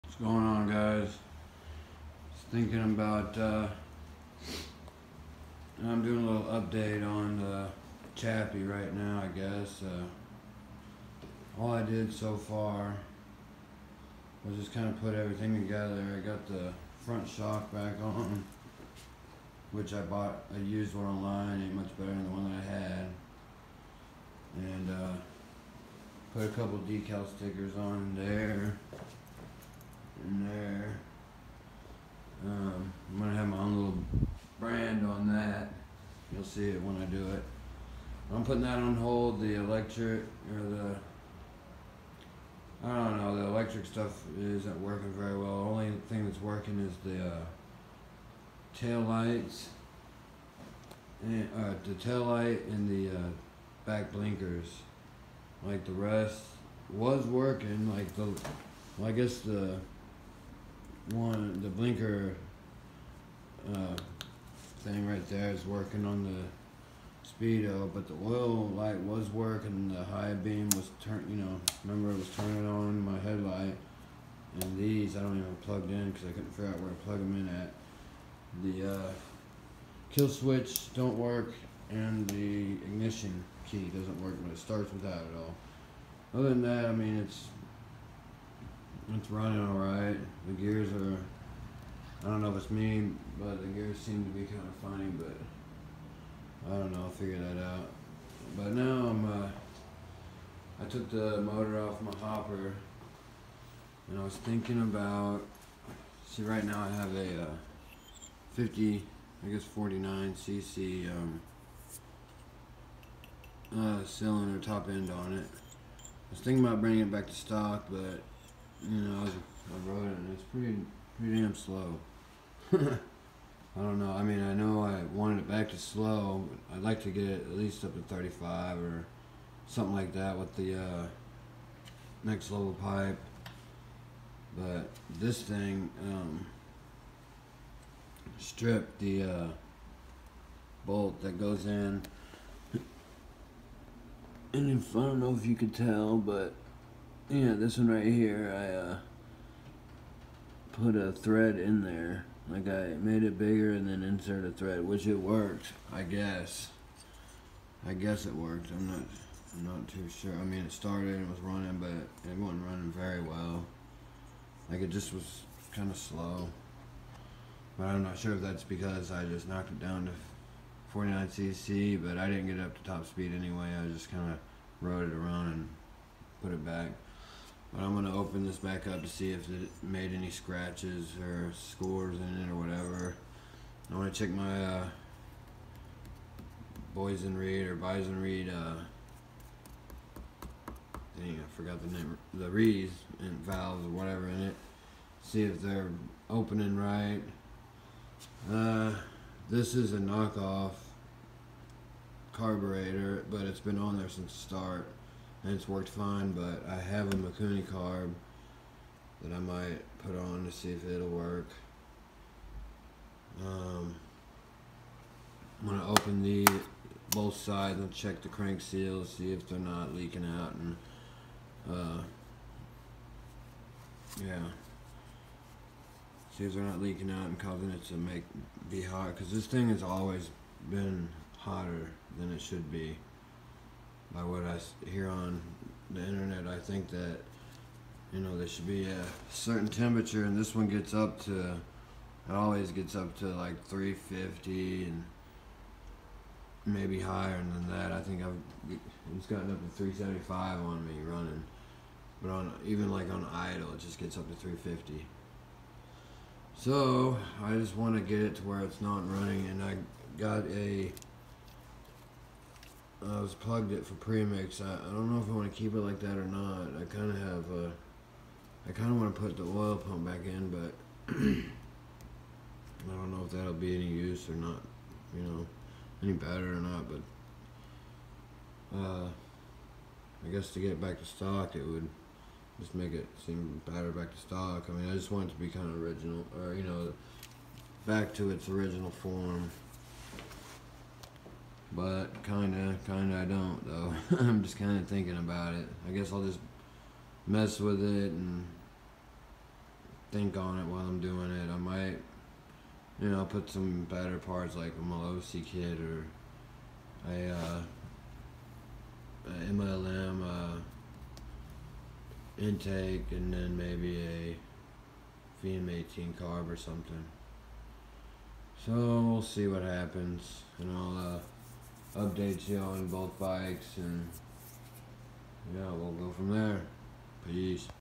Yamaha qt50 upkeep
Wondering if I should upgrade to keep cool while 60cc or go down to stock with it. Trouble shooting and matainance